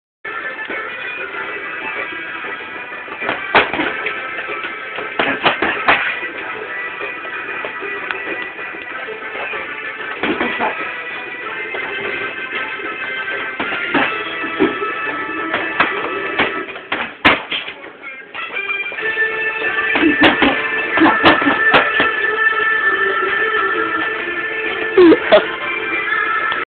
Ricky "The Weeman" Watkins
Rich wacking a punchbag

watch out hatton